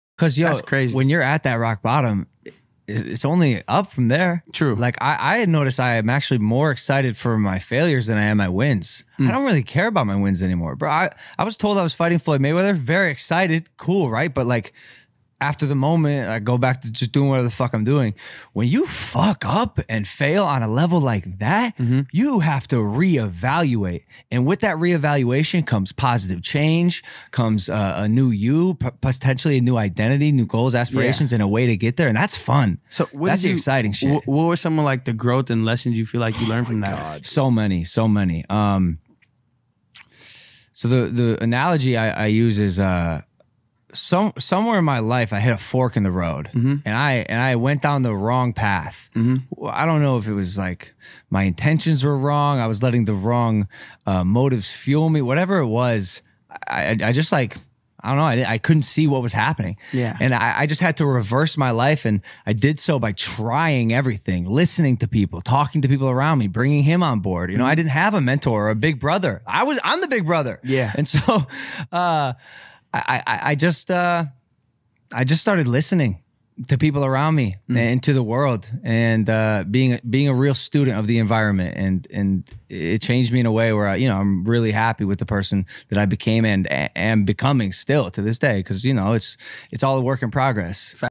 LOGAN PAUL TRENDS ON TWITTER BECAUSE OF THIS VIDEO ABOUT HIS REDEMPTION
Best Clips from around the internet... updating you on the latest news